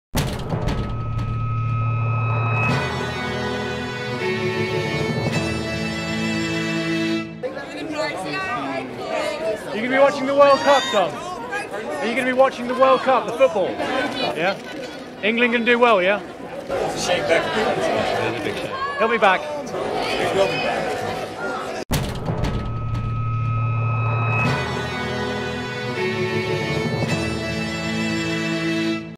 Tom Cruise on David Beckham: "He'll be back"
Tom Cruise chats (briefly!) to Adrian Hieatt from Absolute Radio about England's chances at the World Cup in South Africa 2010, and his superstar friend David Beckham. The interview was filmed at London's National Movie Awards. For more great video interviews check out absoluteradio.co.uk